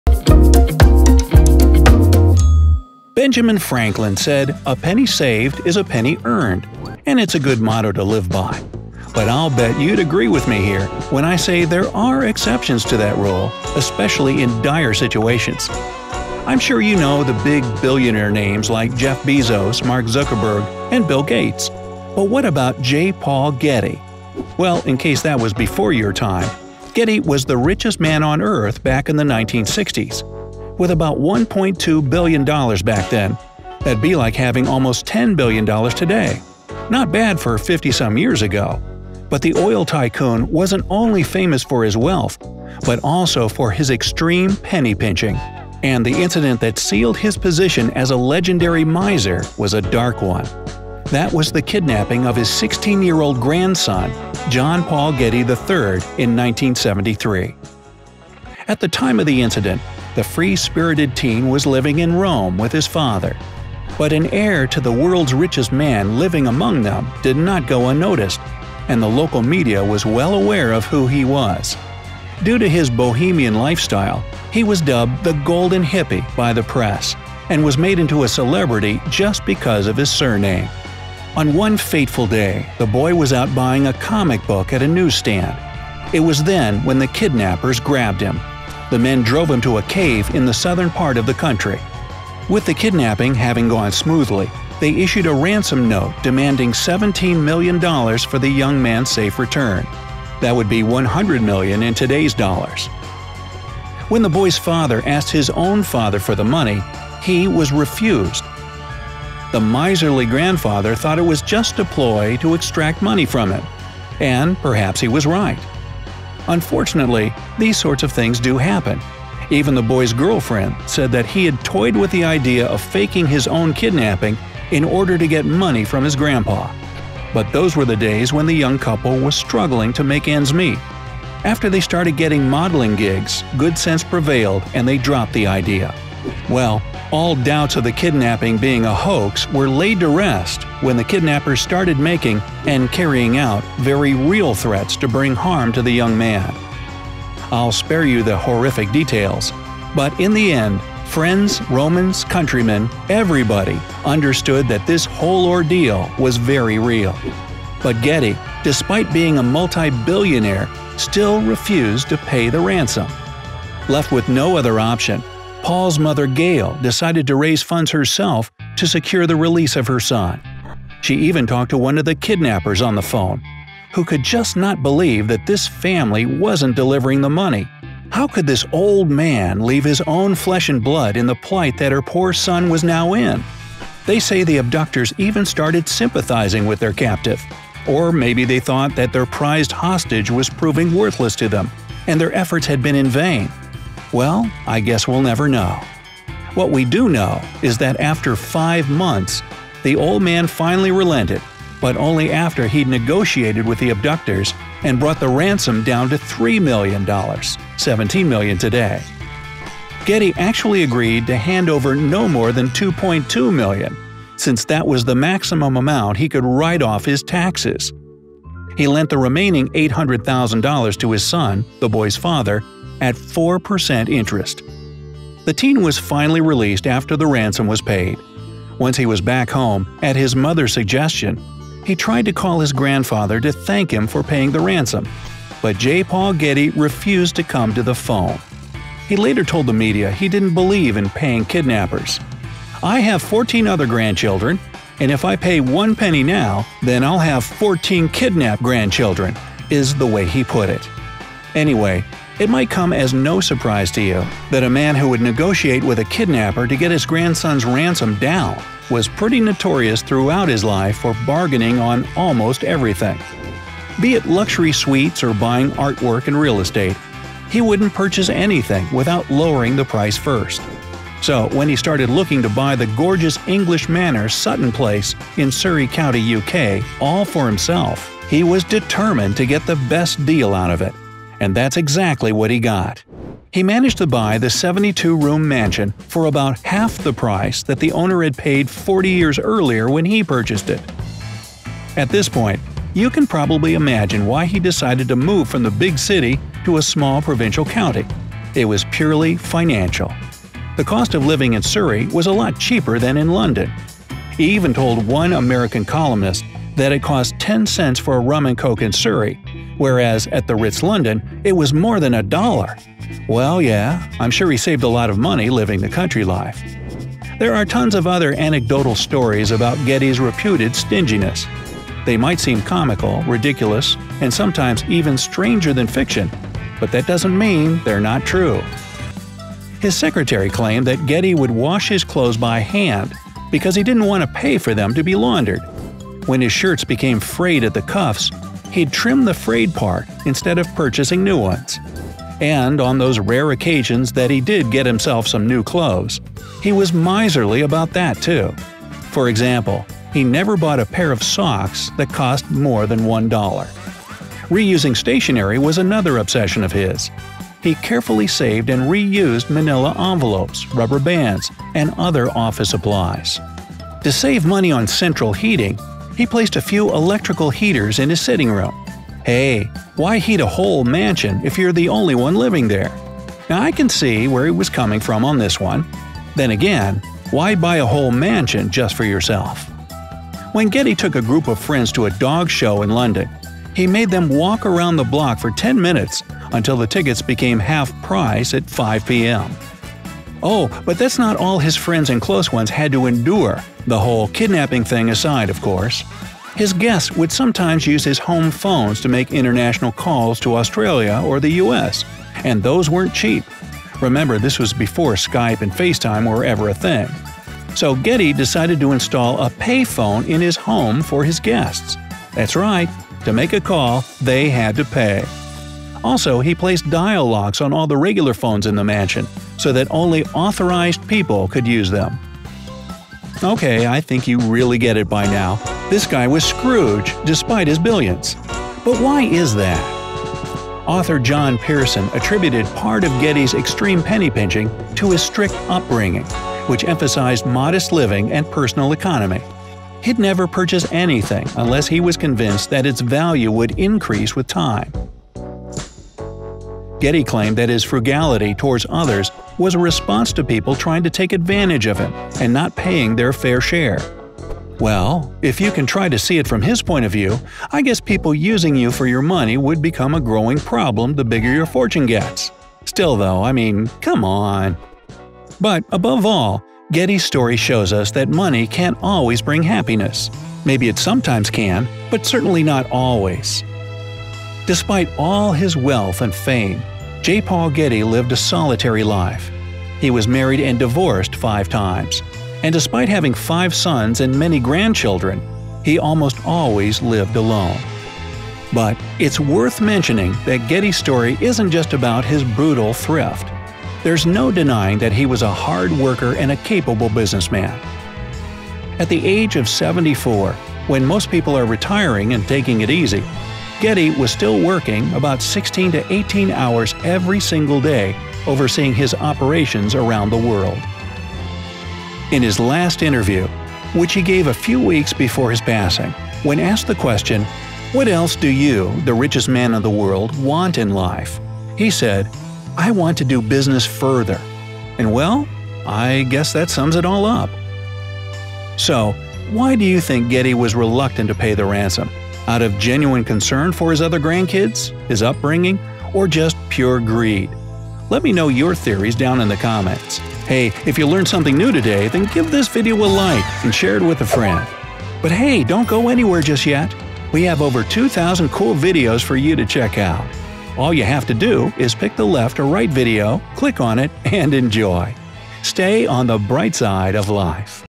The Richest Man Refused to Pay for His Grandson
Benjamin Franklin said a penny saved is a penny earned, and it’s a good motto to live by! But there are exceptions to that rule, especially in tough situations! Everyone knows the big billionaire names like Jeff Bezos, Mark Zuckerberg, and Bill Gates. But what about J. Paul Getty?

Getty was the richest man on Earth back in the 1960s. With about $1.2 billion then, that’d be like having almost $10 billion today! But the oil tycoon wasn’t only famous for his wealth but also for his extreme penny-pinching. And the incident that sealed his position as a legendary miser was a dark one. That was the kidnapping of his 16-year-old grandson, John Paul Getty III, in 1973…

TIMESTAMPS:
$17 million for my grandchild's safe return? 0:46
...Nah, thanks 1:44
How the teen was finally released 2:43
The 72-room mansion for half the price 4:24
Laundry is way too expensive 5:29
A pay-phone for guests. Why not? 6:36
Why was he so miserly? 7:03

Preview photo credit:
This is an Oct. 1966 photo of oil baron J. Paul Getty: By Associated Press/East News,
Billionaire J. Paul Getty exchanges smiles with Tricia Nixon Cox, former President Richard Nixon's daughter, in London on Dec. 15, 1972. The occasion was a party marking Getty's 80th birthday: By Associated Press/East News,

SUMMARY:
- On one fateful day, the boy was out buying a comic book at a newsstand. It was then when the kidnappers grabbed him. They issued a ransom note demanding $17 million for the young man’s safe return.
- When the boy’s father asked his own father for the money, he was refused. The miserly grandfather thought it was just a ploy to extract money from him.
- Well, all doubts of the kidnapping being a hoax were laid to rest when the kidnappers started making very real threats to bring harm to the young man.
- After five months, the old man finally relented but only after he’d negotiated with the abductors and brought the ransom down to $3 million.
- He later told the media that he didn’t believe in paying kidnappers. “I have 14 other grandchildren, and if I pay one penny now, then I’ll have 14 kidnapped grandchildren,” is the way he put it.
- He managed to buy the 72-room mansion for about half the price that the owner had paid 40 years earlier when he purchased it.
- His secretary claimed that Getty would wash his clothes by hand because he didn't want to pay for them to be laundered.
- And on those rare occasions that he did get himself some clothes, he was miserly about that too. For example, he never bought a pair of socks that cost more than $1.
- To save money on central heating, he placed a few electrical heaters in his sitting room.
- Getty decided to install a pay-phone in his home for his guests. That’s right, to make a call, they had to pay!
- Getty claimed that his frugality towards others was a response to people trying to take advantage of him and not paying their fair share.
- Despite all his wealth and fame, J. Paul Getty lived a solitary life. He was married and divorced five times. And despite having five sons and many grandchildren, he almost always lived alone.